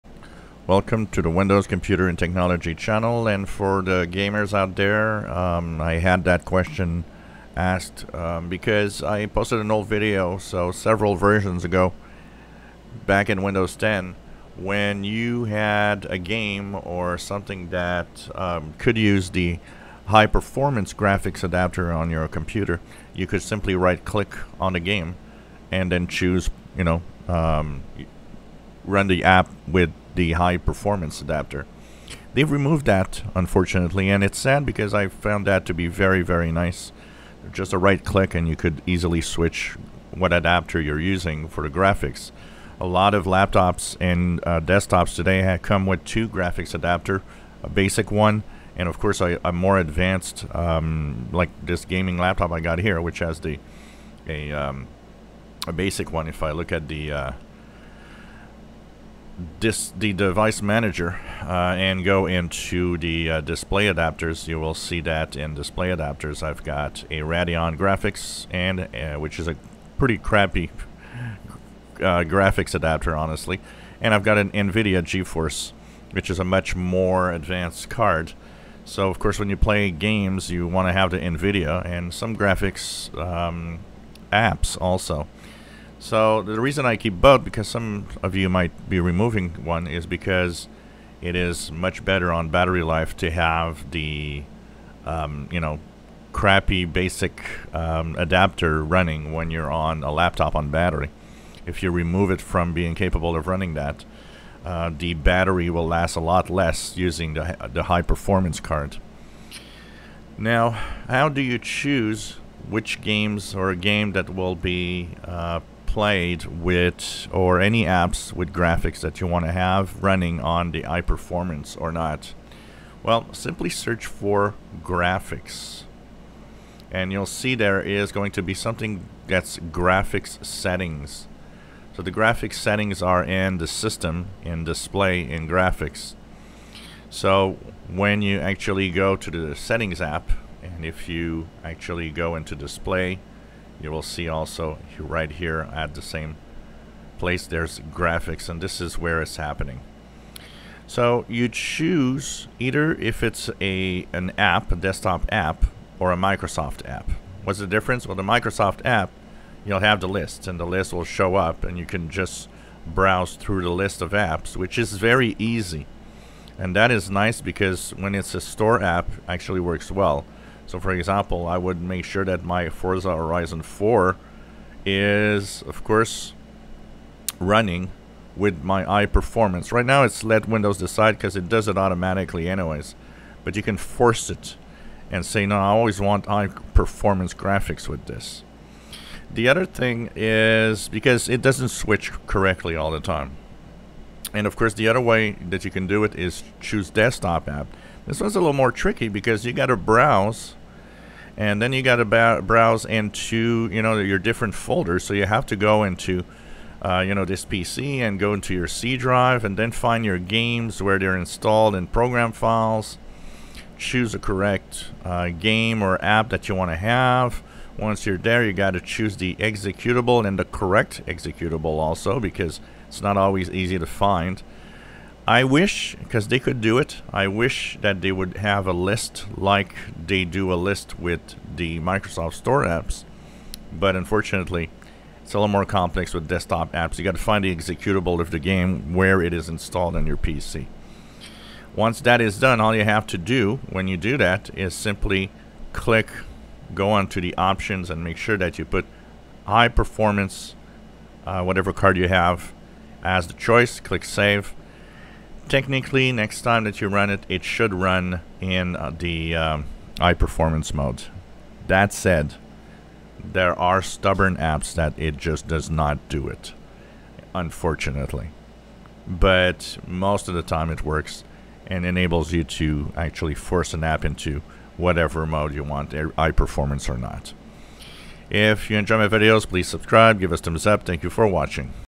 Windows 11 How to choose high performance graphics for games and apps
If you have more than one graphics adapter you might want to choose which one is used, specially the High Performance adapter when you play games or have graphics intensive apps